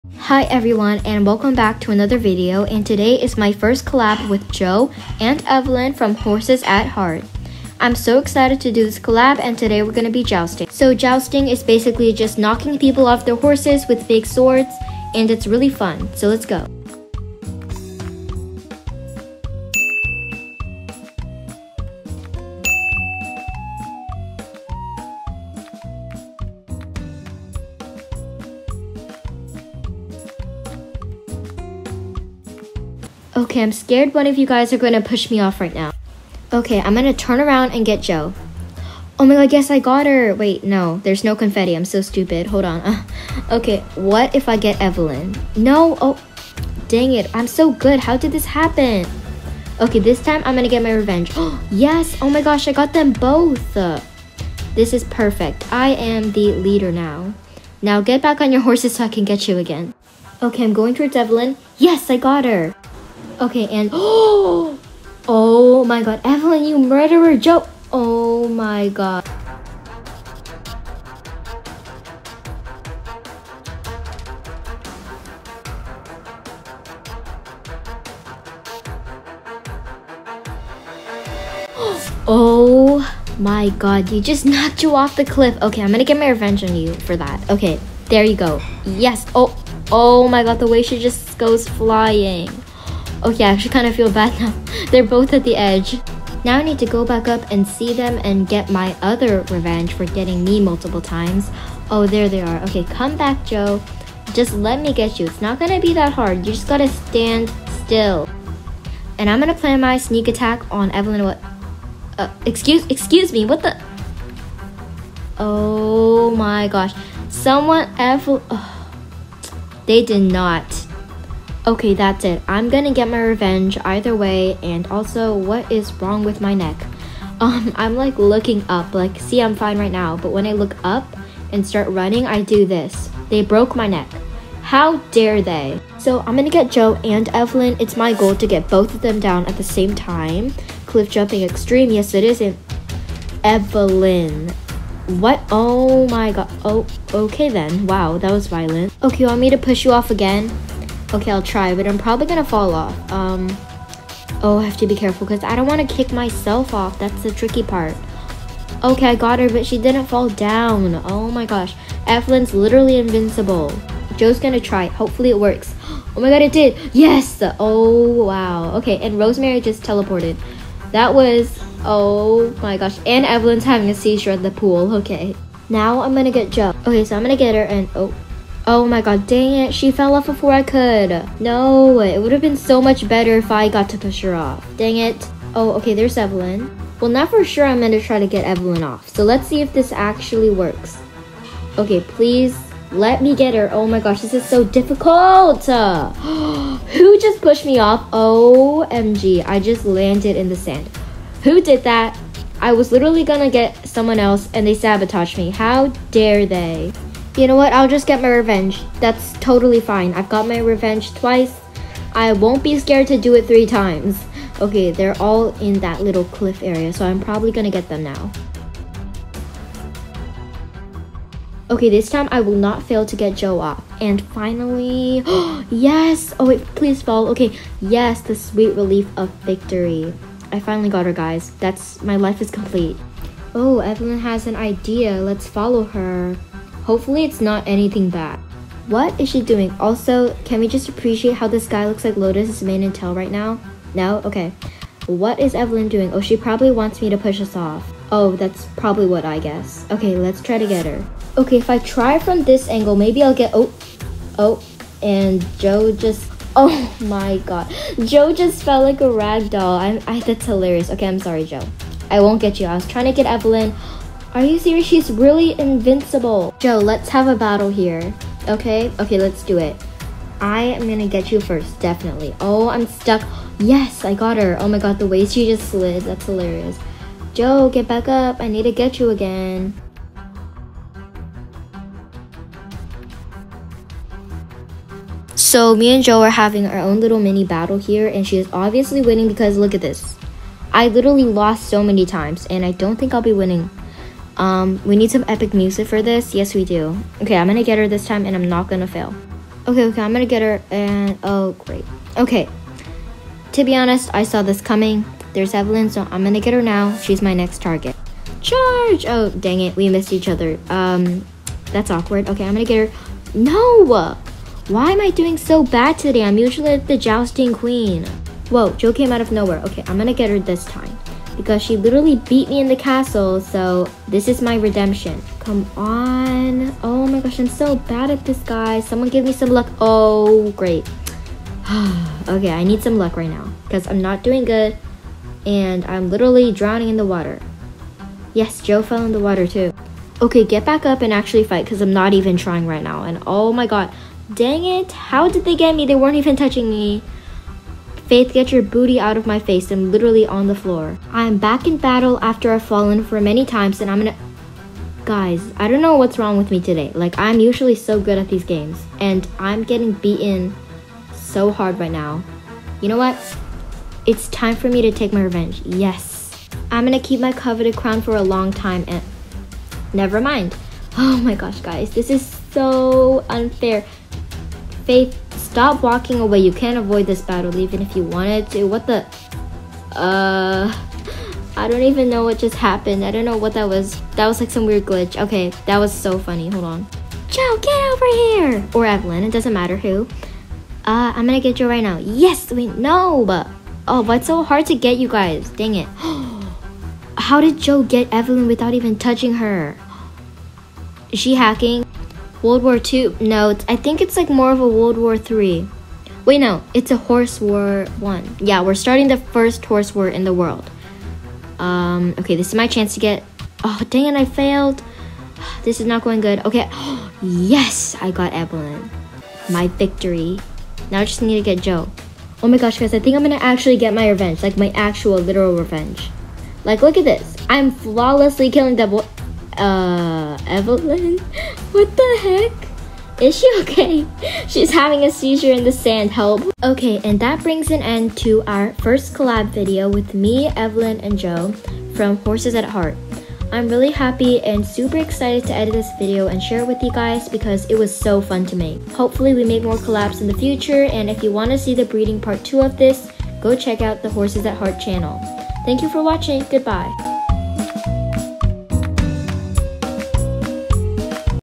JOUSTING In the Medieval Realms.. Collab with @Jo-EvePlayzz ! • PART 1 / PART 2
My first collab! Woo! Honestly, I never knew I would collaborate with anyone until now lol
It was so fun to make! This is part one out of two parts, the other part will
be shortly posted on Joe and Evelyn’s channel.